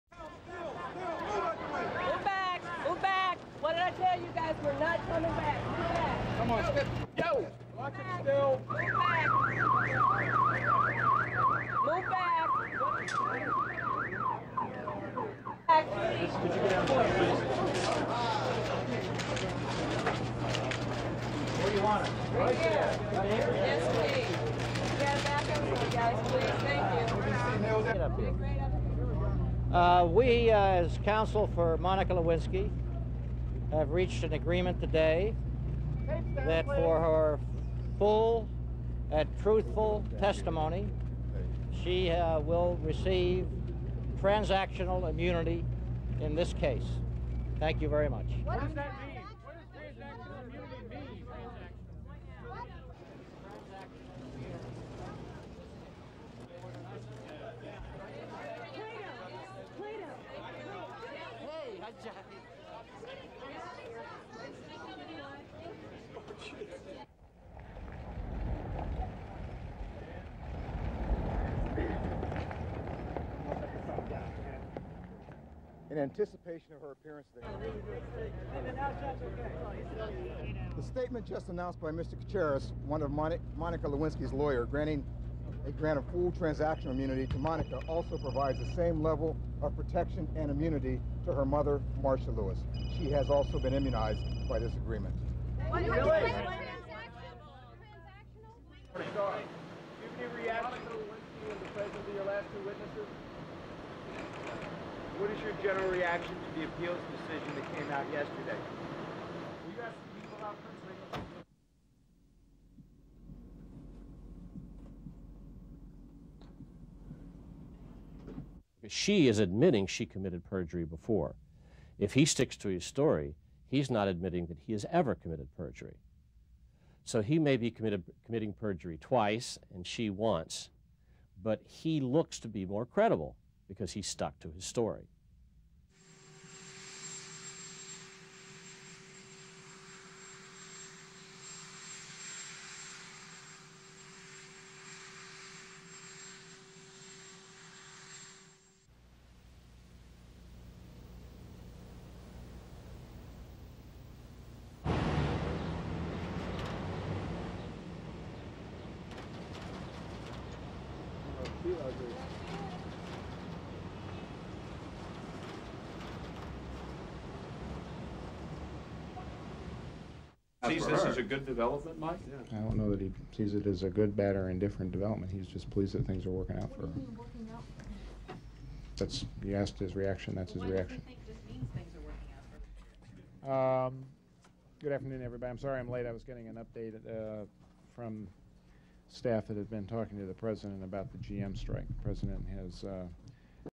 USA: DEAL REACHED IN CLINTON/LEWINSKY ALLEGED SCANDAL CASE (2)
Former Whitehouse intern Monica Lewinsky has been given blanket immunity in exchange for testimony in the six-month investigation of her relationship with President Clinton.

Ms. Lewinsky's mother, Marcia Lewis, was also granted the same level of protection from prosecution.

The mother had frequent telephone conversations with her daughter about Ms. Lewinsky's relationship with Clinton, according to 20 hours of tape recordings between Ms. Lewinsky and former friend Linda Tripp.

A horde of cameras greeted Monica Lewinsky when she got out of a taxi in front of her lawyers' office in Washington.

The former White House intern waded through the throng of reporters without comment

A short time later her attorneys Plato Cacheris, Jacob Stein and Nathaniel Speights came out and announced the deal.

SOUNDBITE:

"We, as counsel for Monica Lewinsky have reached an agreement today, that for her full truthful testimony, she will receive transactional immunity in this case. Thank you very much."
SUPER CAPTION: Plato Cacheris, Monica Lewinsky's Attorney

Transactional immunity means that Lewinsky will not be prosecuted for any testimony that she gives to Starr's office.

It is the most protective type of immunity that a witness can get.

Billy Martin, the attorney for Lewinsky's mother, said the same level of protection has been granted to Marcia Lewis.

Lewis had frequent telephone conversations with her daughter about her relationship with Bill Clinton, according to tape recorded conversations between Lewinsky and Linda Tripp.

SOUNDBITE:

"The statement just announced by Mr Cacheris, one of Monica Lewinsky's lawyers, granting a full transactional immunity to Monica also provides the same level of protection and immunity  to her mother Marcia Lewis, she has also been immunised by this agreement."
SUPER CAPTION: Billy Martin, Marcia Lewis' Attorney

Legal sources said after talking to prosecutors for five hours in New York on Monday, Lewinsky has agreed to testify that she had a sexual relationship with the president.

This is something they have both denied under oath.

Lewinsky's account also is said to include information relevant to Starr's probe of possible obstruction of justice by Clinton and his associates in the Paula Jones' sexual harassment case.

While Lewinsky's immunity deal may make some political trouble for Clinton, law professor Paul Rice says it shouldn't be much of a legal problem.

Rice says Clinton simply has to repeat his earlier denials, then it's Lewinsky's word against the president's.

SOUNDBITE:

"She is admitting she committed perjury before, if he (Clinton) sticks to his story he's not admitting that he's ever committed perjury. So he may be committing perjury twice, she once, but he looks to be more credible, because he stuck to his story."
SUPER CAPTION: Paul Rice, Law Professor, American University

The immunity deal comes a day after Clinton was handed a major defeat.

Monday, a federal court of appeals panel rejected Clinton's claim of attorney-client confidentiality and ordered presidential adviser Bruce Lindsey to answer questions before a grand jury.

The White House can still appeal the decision.

Meanwhile, Lewinsky's former friend Linda Tripp is back before the federal grand jury for a seventh day.

It was Tripp whose secret tape recordings triggered the criminal investigation of the president.

The Whitehouse was being pretty non-committal about the latest development in the case.

SOUNDBITE:

SUPER CAPTION: Mike McCurry, White spokesman